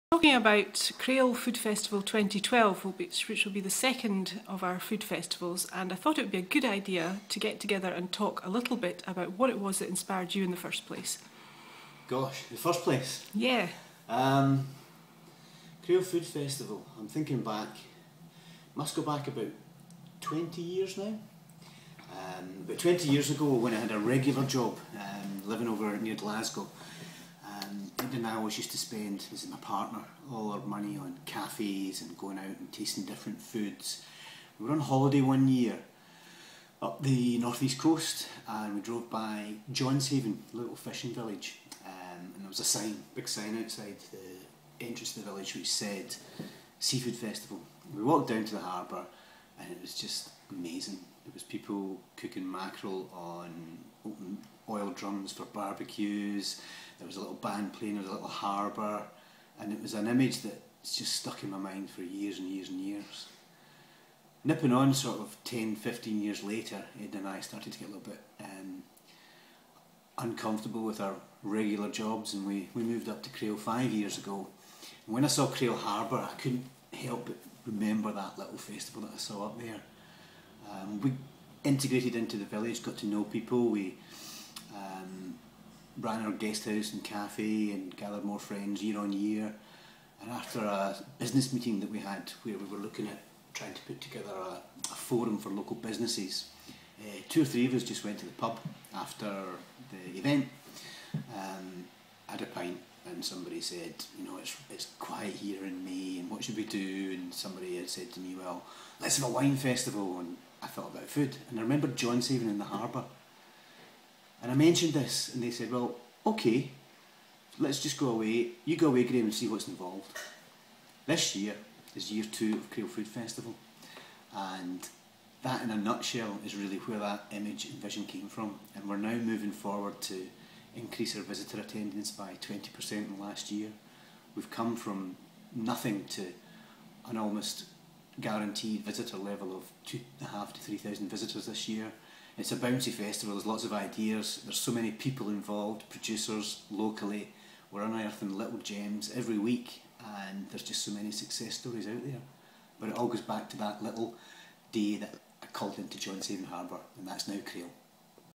Crail Food Festival Inspiration
Graham Anderson of the Honeypot Guest House and Tearoom in Crail tells us how a visit to Johnshaven inspired his dream to start a Food Festival in the village of Crail.